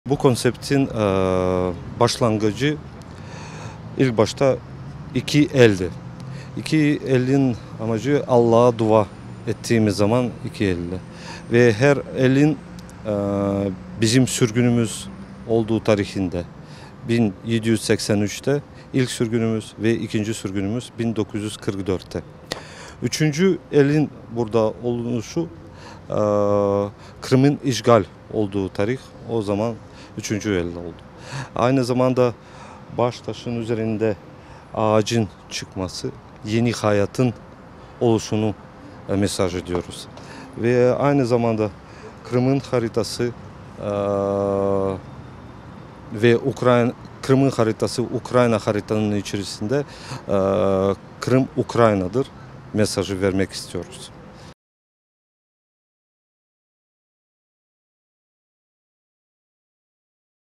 Karayev: Anıt, Kırım Tatar halkının tarihindeki 3 büyük trajediyi yansıtıyor
Kırım Cephesi Girişimi Koordinatörü Feliks Karayev, Kırım Haber Ajansına bugün Kıyiv’de açılan Kırım Tatar Soykırımı Kurbanları Anıtı'nın anlamını anlattı.